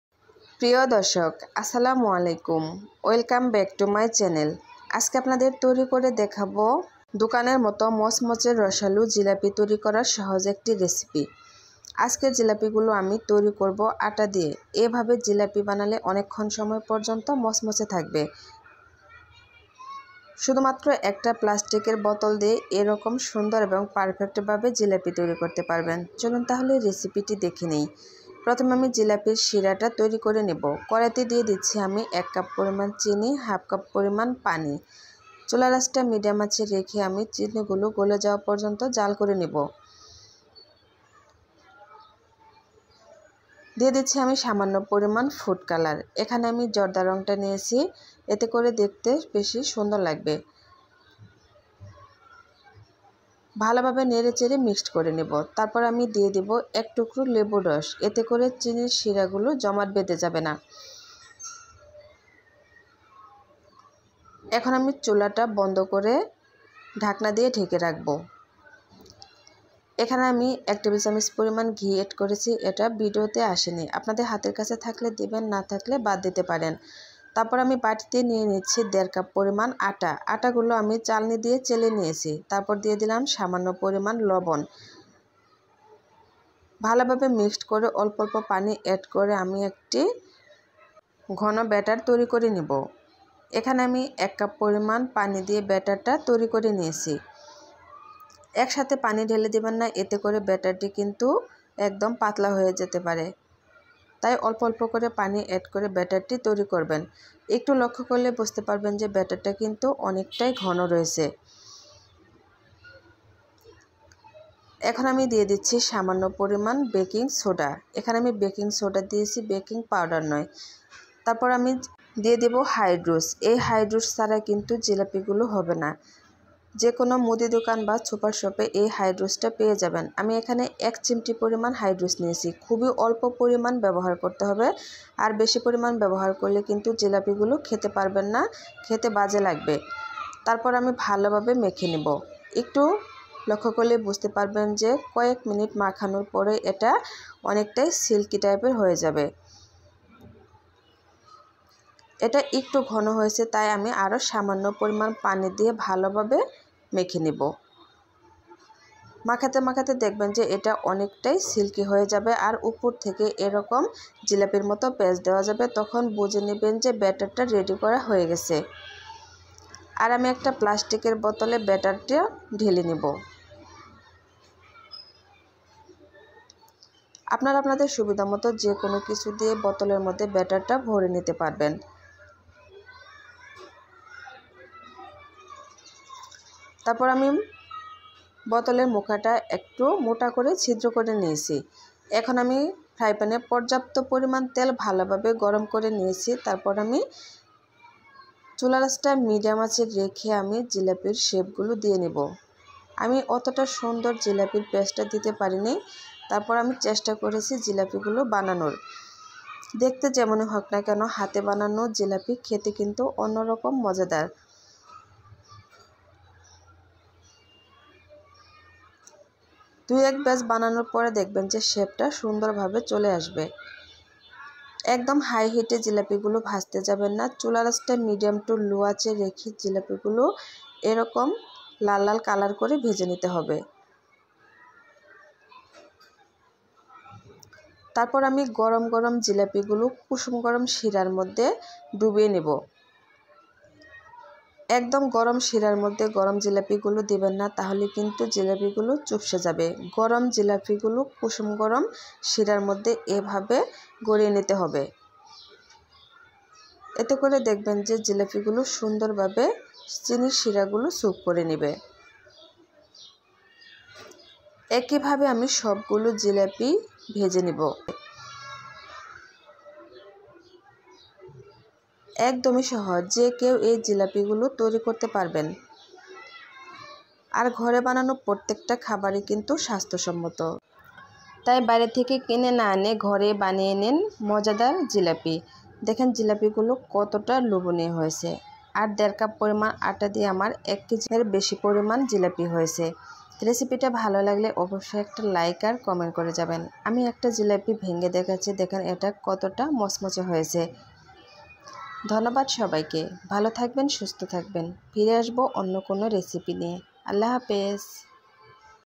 দুনিয়ার সবচেয়ে সহজ রেসিপিতে আটা দিয়ে দোকানের মতো মচমচে রসালো জিলাপি রেসিপি।Perfect Jilapi Recipe.
আটা দিয়ে দোকানের মতো মচমচে রসালো জিলাপি বানানোর সহজ রেসিপি